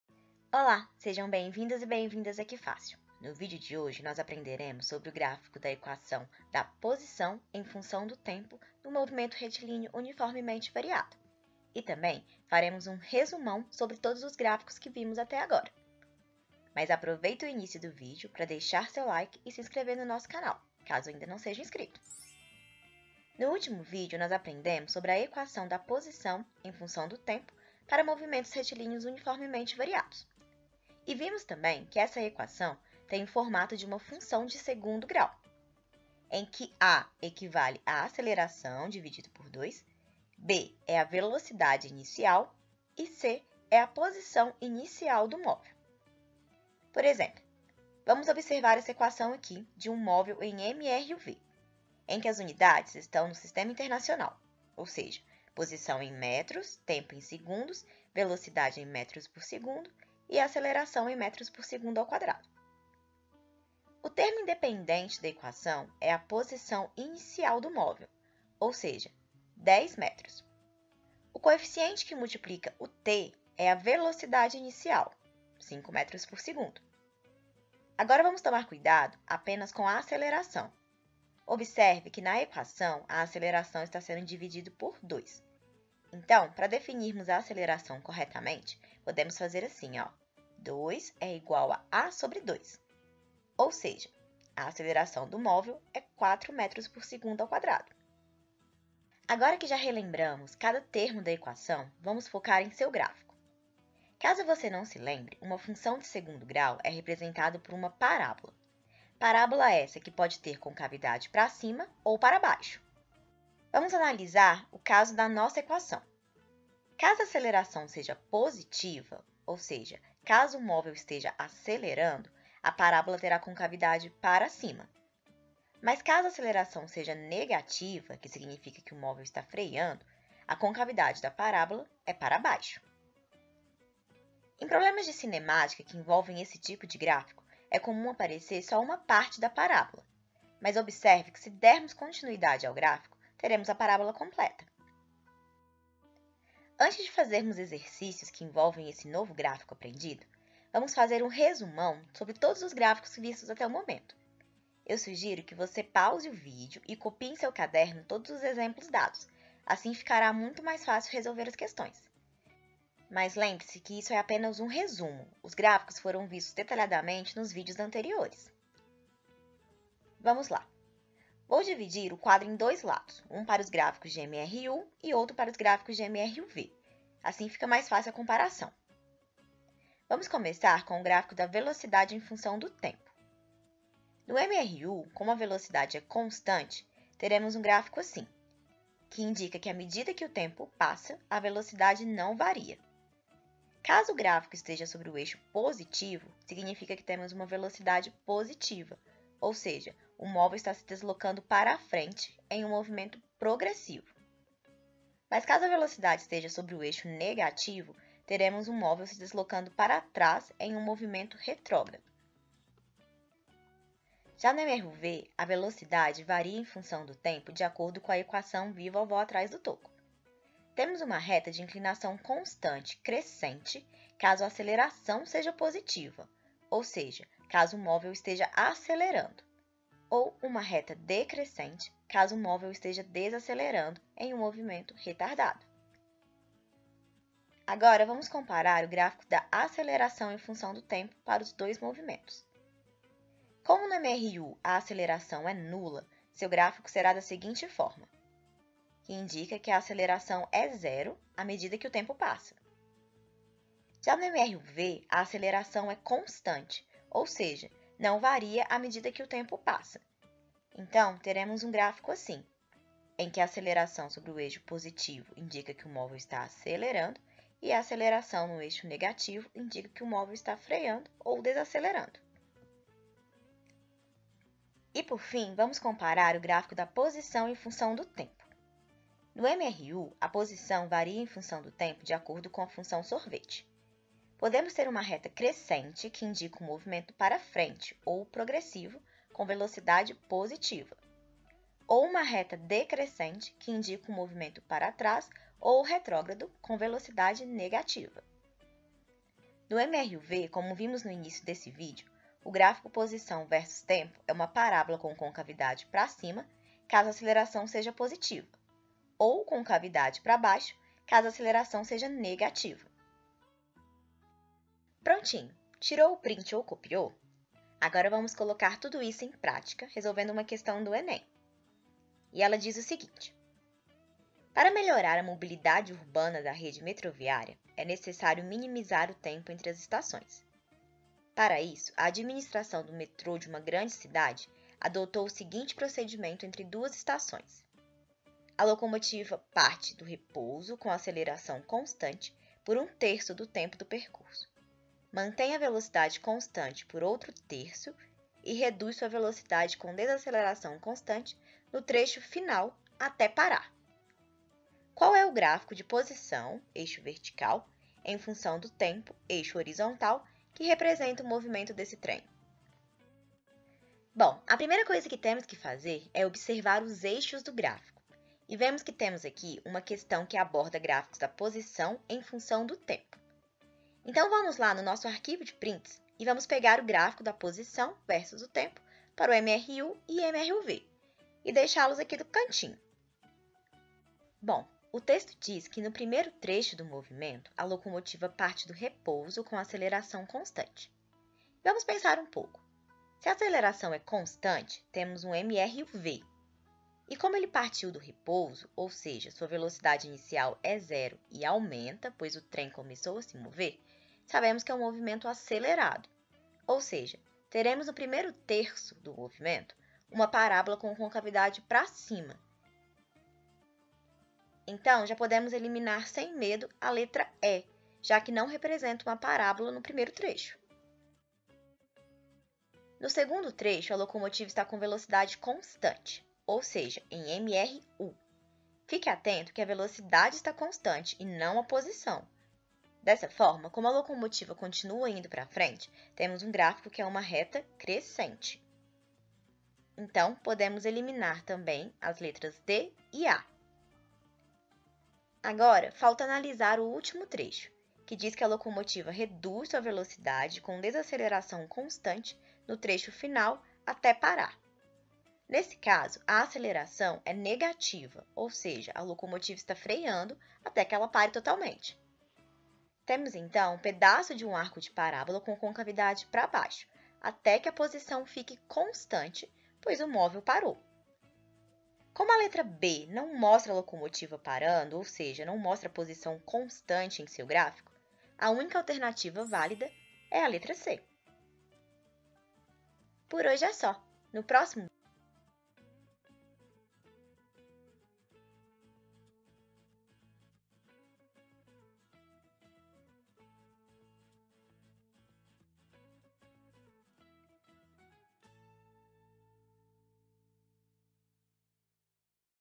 Gráficos de MRU e MRUV - Resumão
No vídeo de hoje nós veremos como é o gráfico da posição em função do tempo para MRUV, e no final, faremos um resumão de todos os gráficos que vimos até agora.